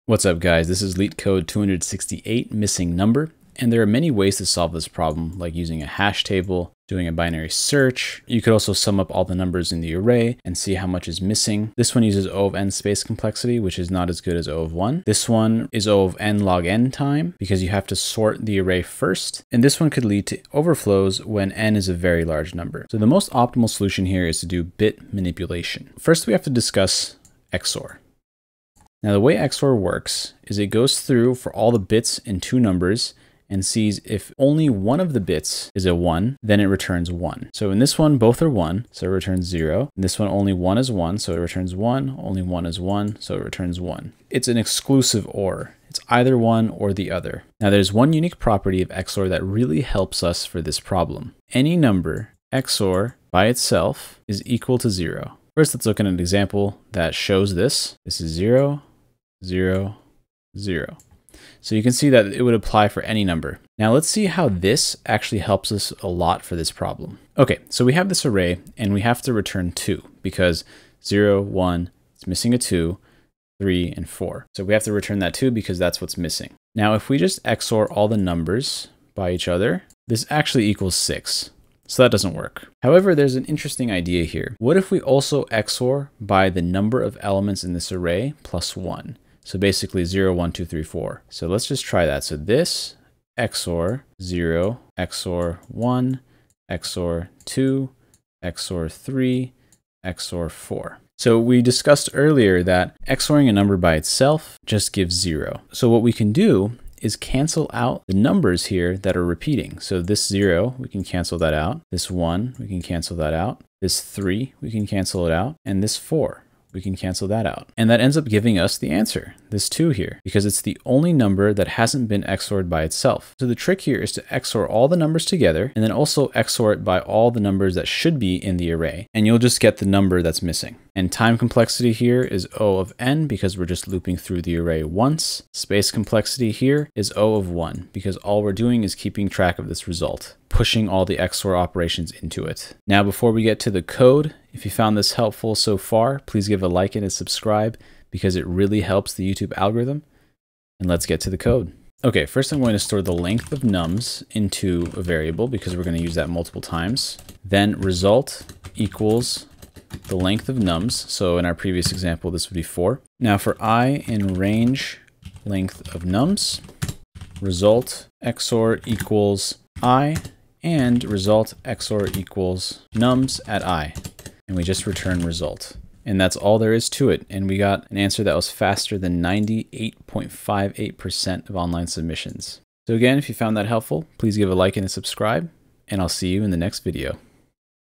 Leetcode Missing Number | Bit Manipulation | Python
00:00 - Intro and Nonoptimal Solutions
00:25 - XOR
00:47 - XOR Unique Property
01:07 - Optimal Solution
02:31 - Code

BOOKS I RECOMMEND:
📖 Clean Code
📖 The Singularity is Near
📖 Deep Work